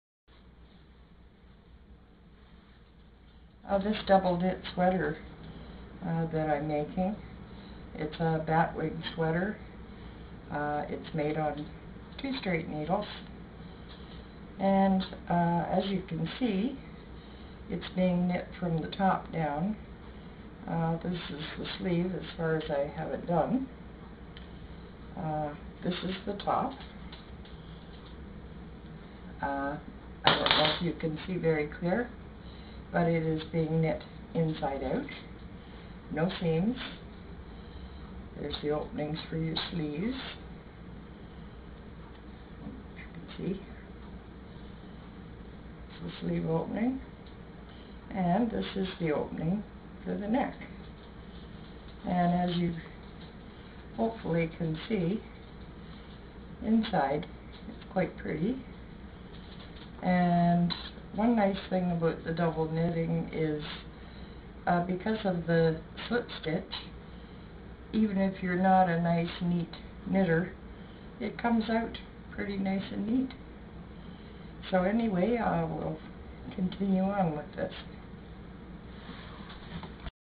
Double knit no seams batwing sweater on two stra
This sweater is double knit on straight needles by Lynn Frederickson.
Starting at the top and knitting down to the ribbed waist line. It is made inside out. When you finish the last row, you turn it right side out and put it on. I will be posting instructions for this sweater in the near future.